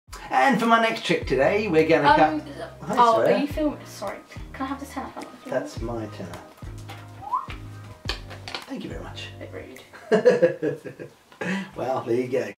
Umm...Can I have this tenner?
Just a fun little effect with a £10 note. He he